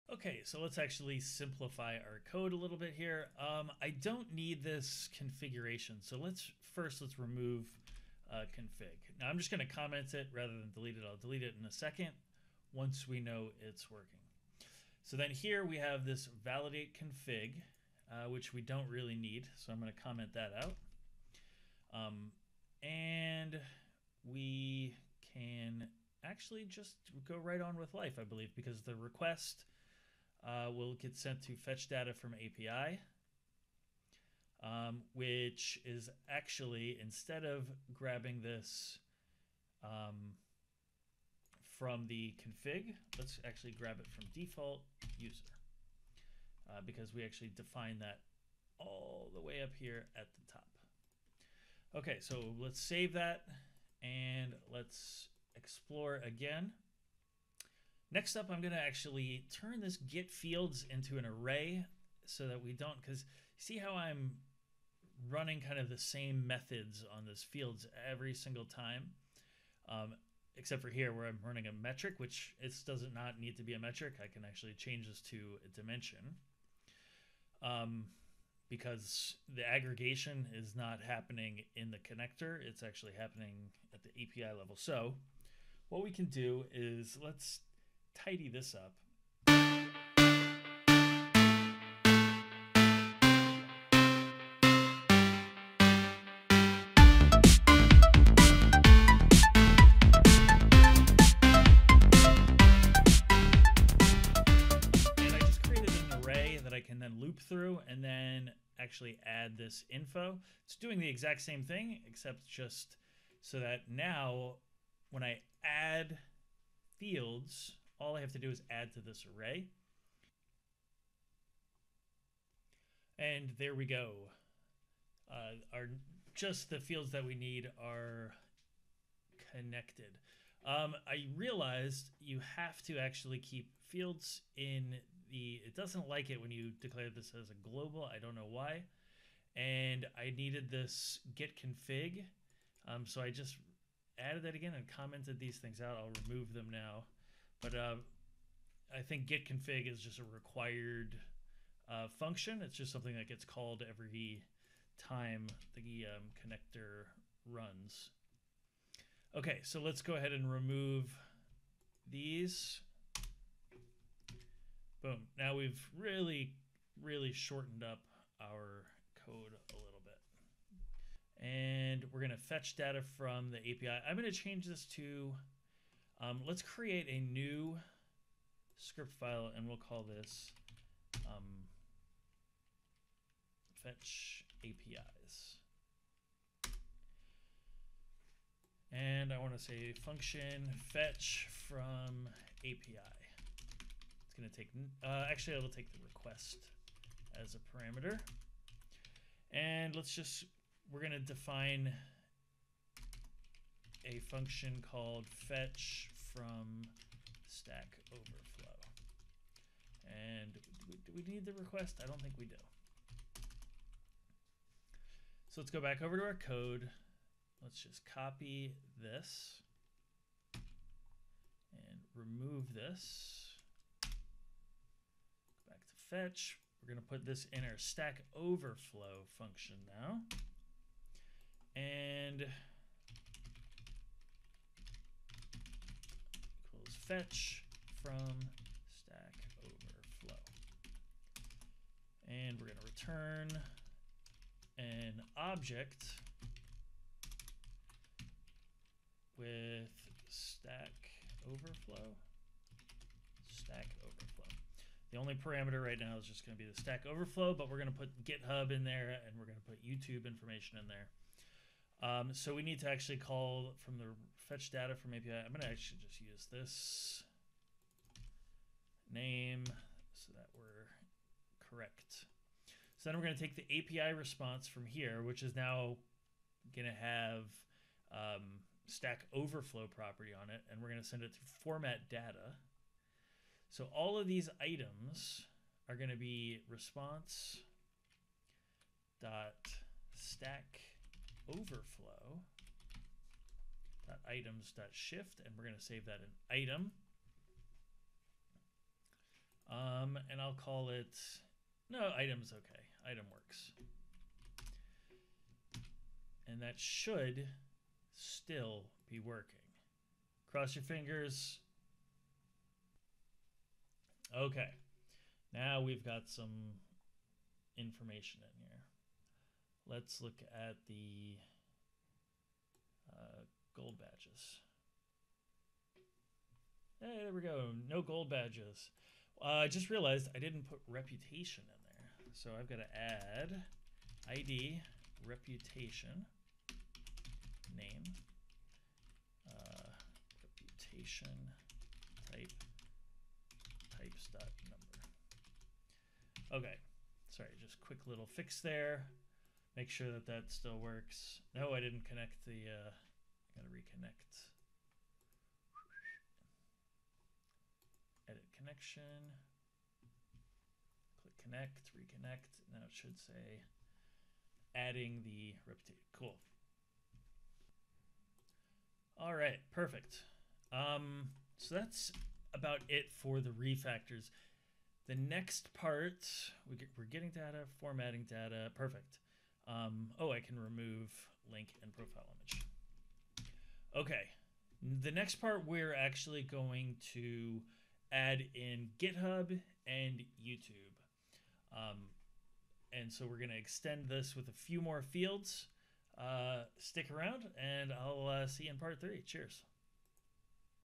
Building a data studio connector Part 2
Introduction to a short series on building a custom data connector in Google's Data Studio